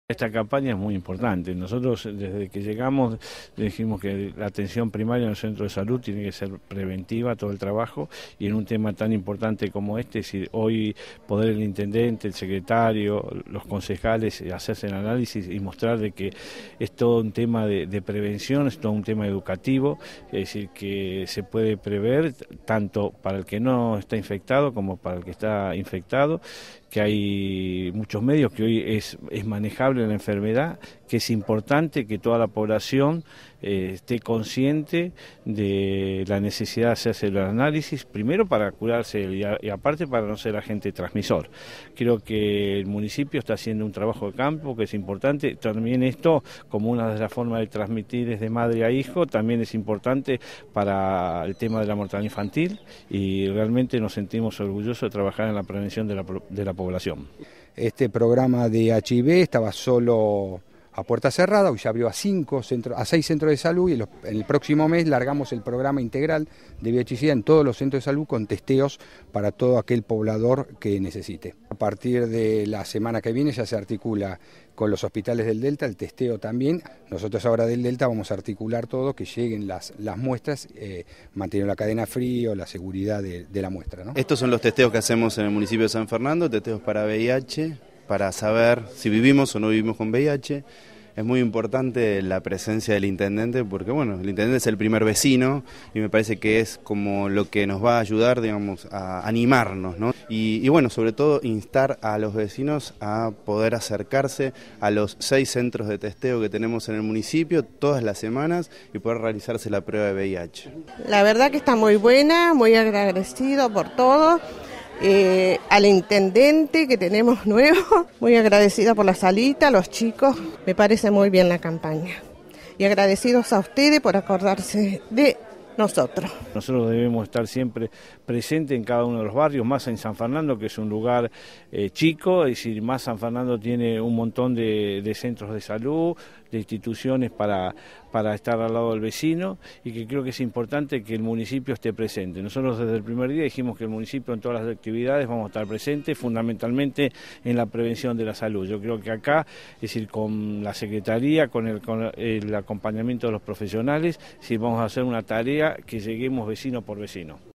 Andreotti: "Realmente nos sentimos orgullosos de trabajar en la prevención de la población"
El intendente Luis Andreotti y el Secretario de Desarrollo Social, Medio Ambiente y Salud Pública, Gustavo Santero, se realizaron el testeo de VIH en el Centro de Salud "Dr. I. Piaggi" ubicado en Málaga y Garibaldi.

En el marco de continuar con las políticas de salud en San Fernando en el día de hoy se lanzó oficialmente el cronograma de Testeos para VIH 2012 que funcionan en los diferentes Centros de Salud del Municipio. El Intendente Luis Andreotti y de Gustavo Santero, Secretario de Desarrollo Social, Medio Ambiente y Salud Pública se realizaron el examen psicológico y el testeo a Director del Programa  Municipal de VIH/Sida.

"Esta campaña es muy importante. Nosotros desde que llegamos al gobierno dijimos que la atención primaria en los Centros de Salud tiene que ser preventiva. Que hoy el Secretario Santero, los concejales y yo podamos realizarnos el examen de VIH, muestra que ésto es una tarea de educación y de prevención tanto para quien está infectado como para quien no lo está. Es importante que toda la población esté consciente de la necesidad de hacerse los análisis, primero para curarse uno y segundo para no ser agente transmisor", sostuvo el Intendente Luis Andreotti.

Con esta iniciativa se sumaron a las actividades del Programa, los concejales Ramón Robles, Leandro Ipuche y Patricia Folino, quien además se sometió al exámen de sangre, al igual que muchos vecinos de San Fernando, convencidos de la importancia de detectar en forma temprana, el virus de la inmunodeficiencia humana (VIH), causante del síndrome de inmunodeficiencia adquirida (SIDA).